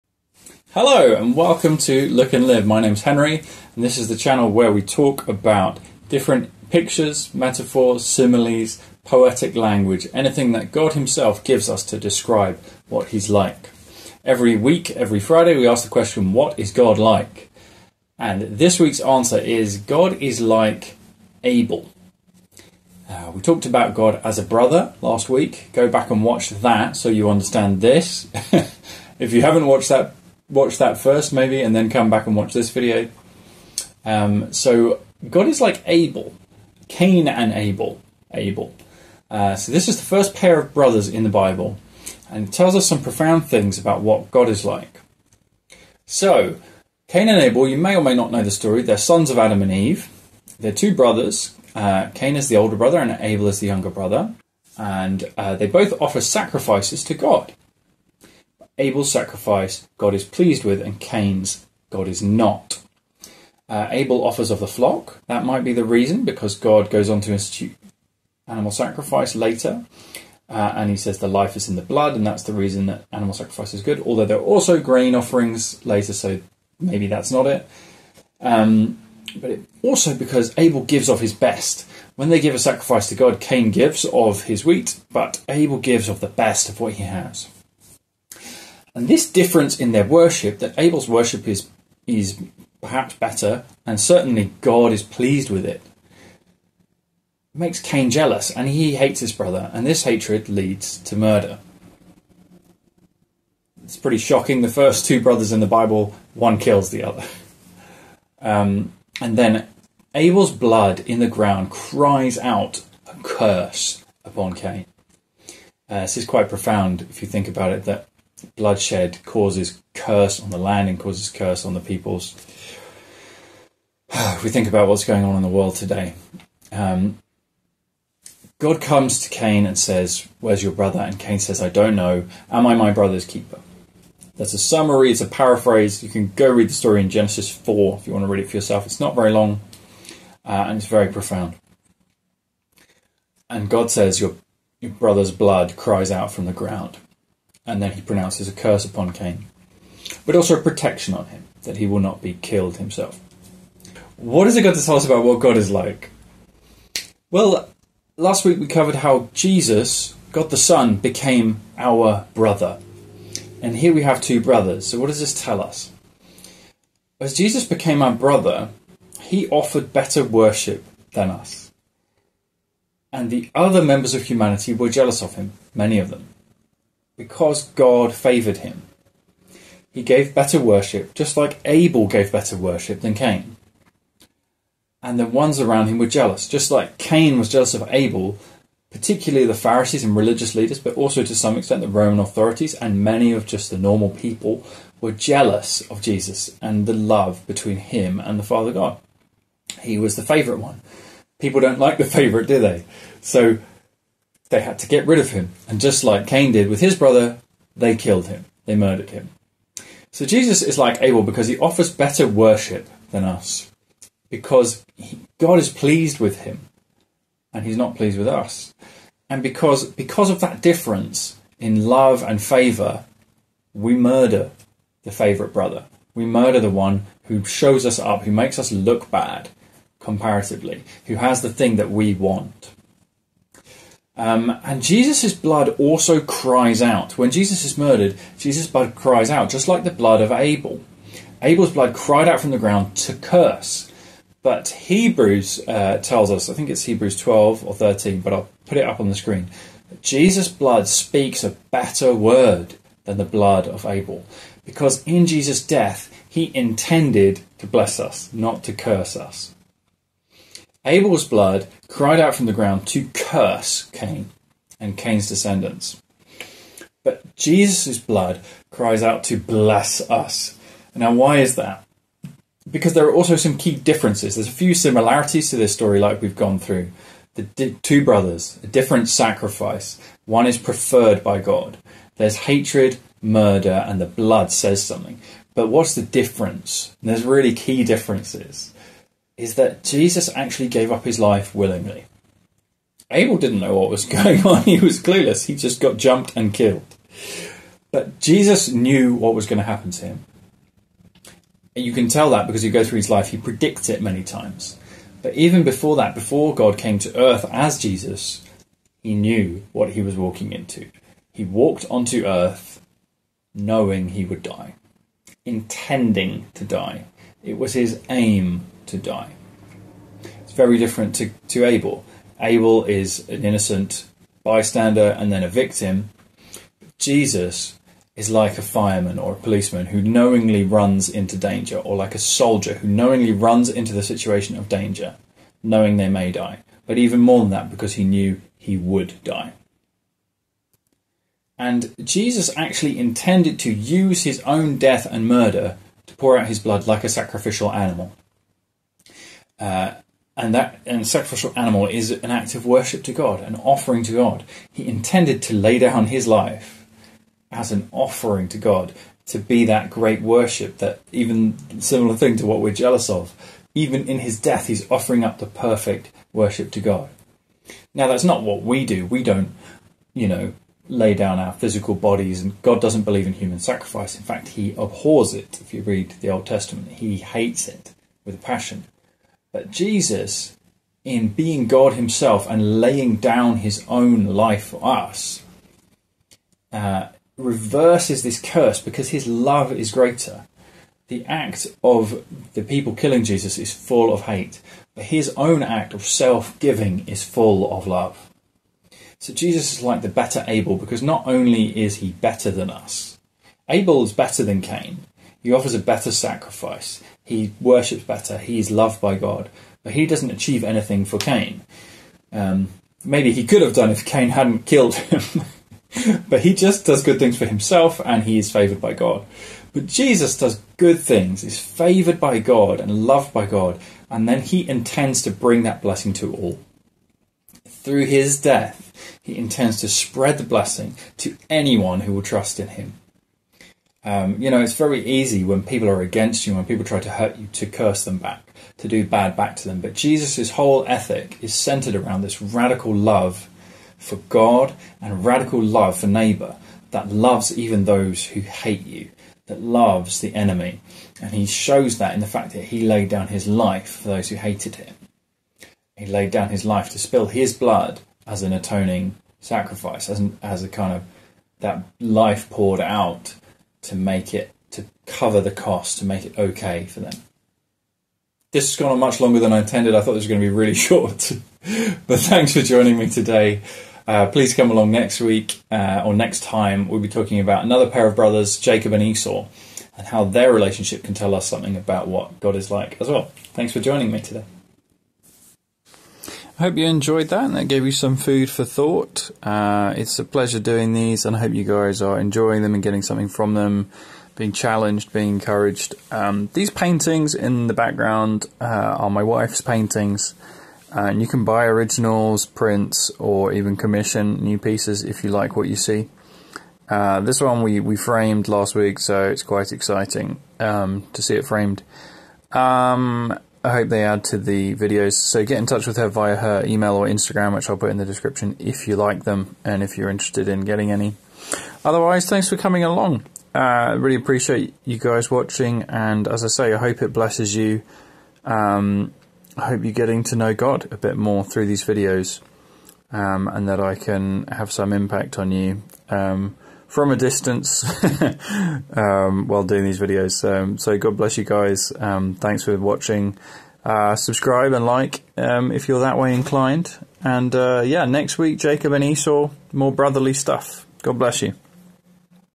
Abel (but Better!) - What is God Like?
What is God like?
God is like Abel: a better brother who offers pure worship, whose blood cries out.
And we are like Cain: a jealous brother who hates and kills.

BUT Jesus' blood blesses us instead of cursing us.

Genesis 4v1+